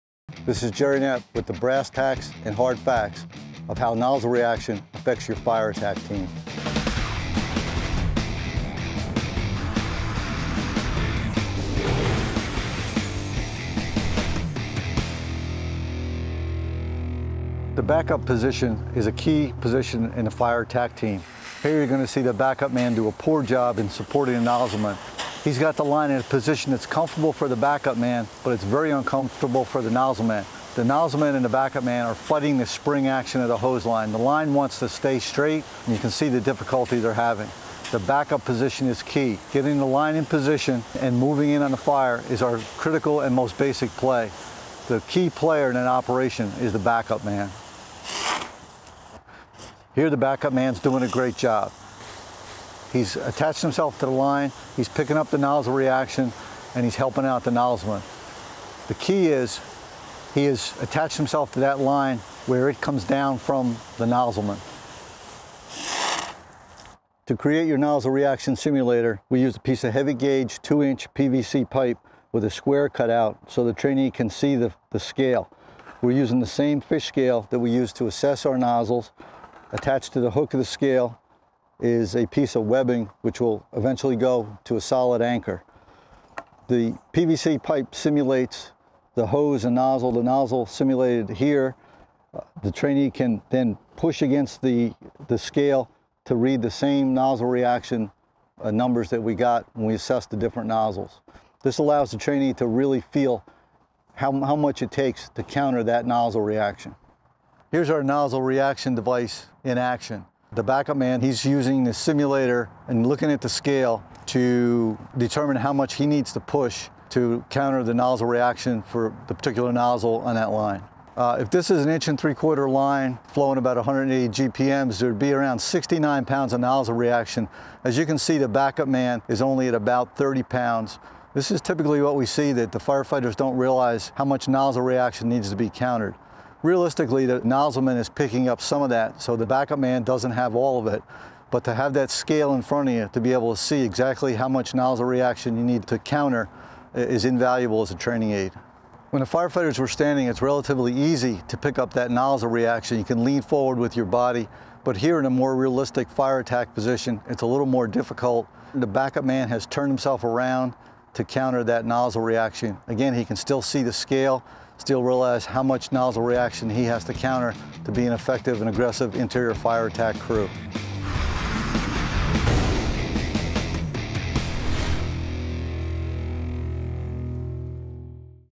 Effects of Nozzle Reaction on Fire Attack Team - Brass Tacks & Hard Facts (Episode 9)
Nozzle Reaction not managed properly can have a great effect upon the fire attack team.

In this video Jerry Knapp shows how the backup firefighter plays a key role in managing nozzle reaction and how to build a simple training aid to help demonstrate this.

This video is based on his October 2007 Fire Engineering article written with Christopher Flatley and Tim Pillsworth titled “Nozzle Reaction Simulator Can Improve Fire Attack Skills”.